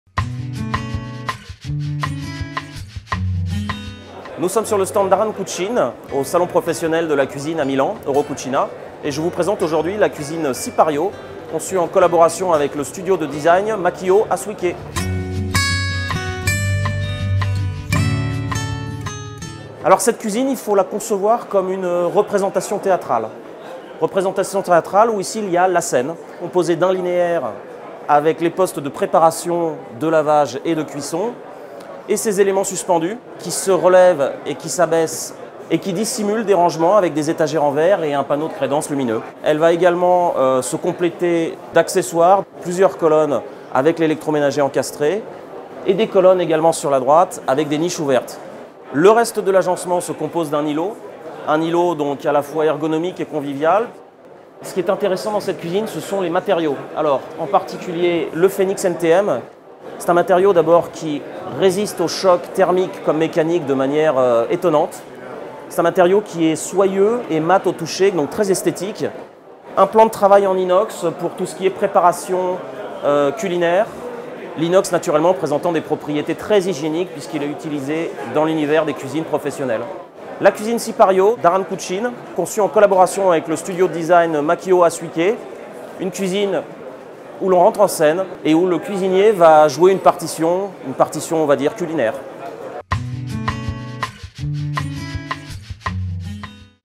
cuisine Sipario
Salon international du meuble de Milan : la cuisine Sipario conçue par le studio de design Makio Hasuike